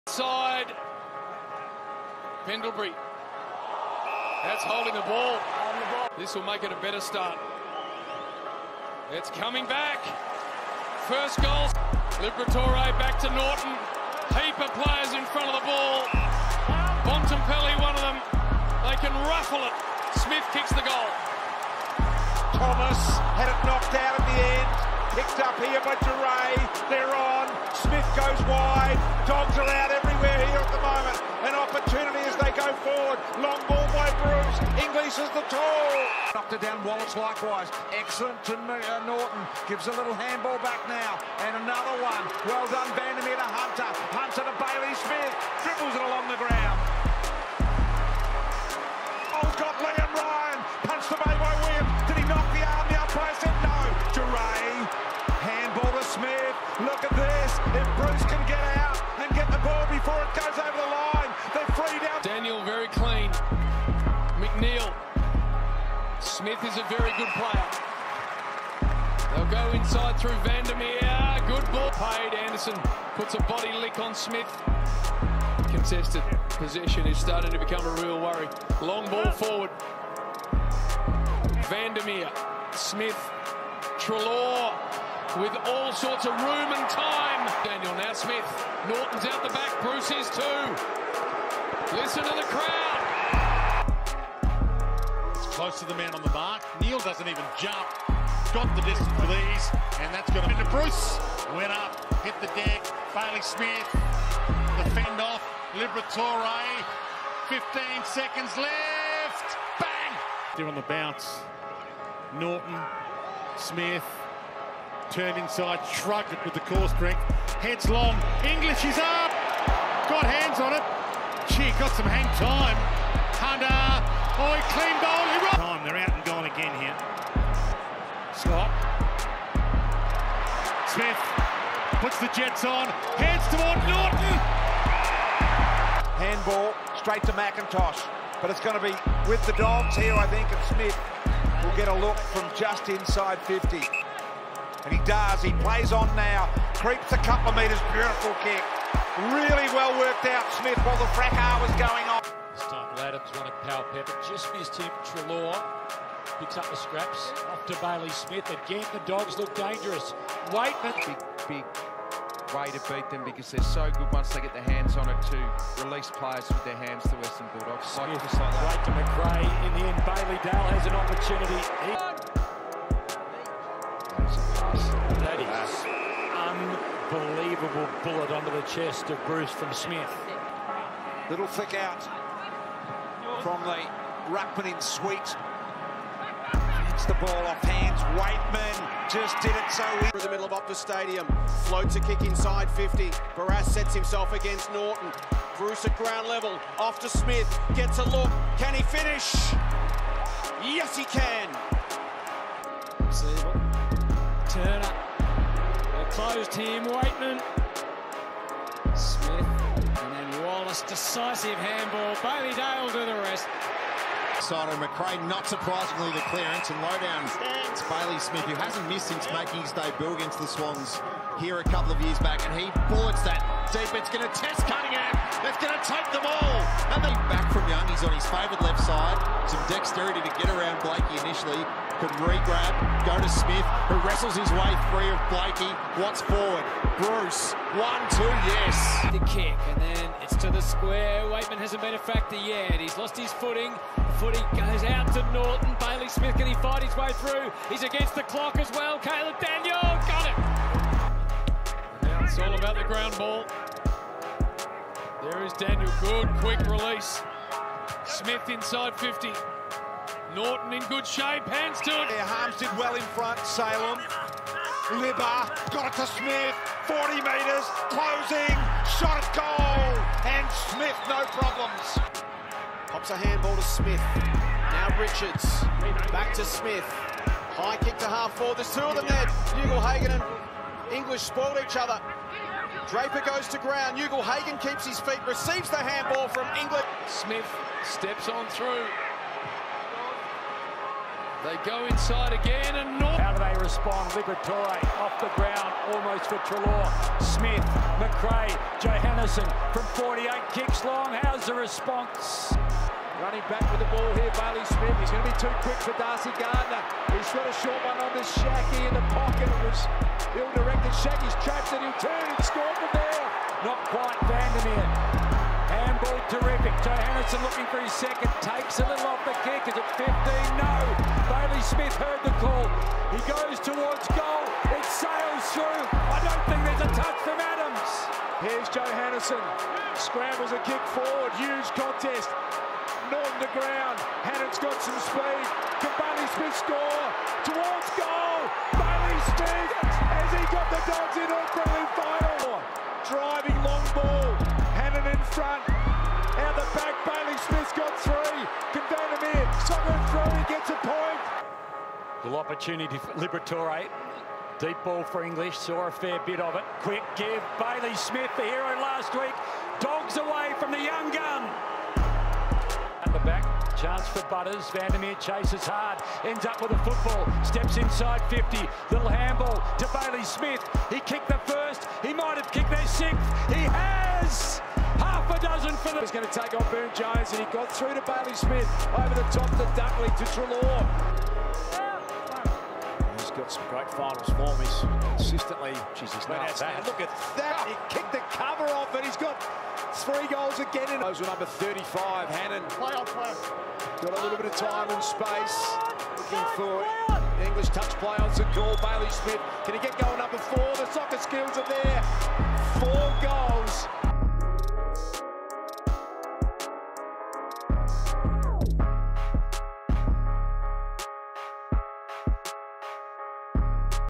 Bailey Smith 2021 Highlight Reel
Games: 26
Averages: 23.6 disposals, 3.7 marks, 3.4 tackles, 3.9 inside 50s, 3 clearances, 0.7 goals per game.

Beat by Unknown Instrumentalz (youtube)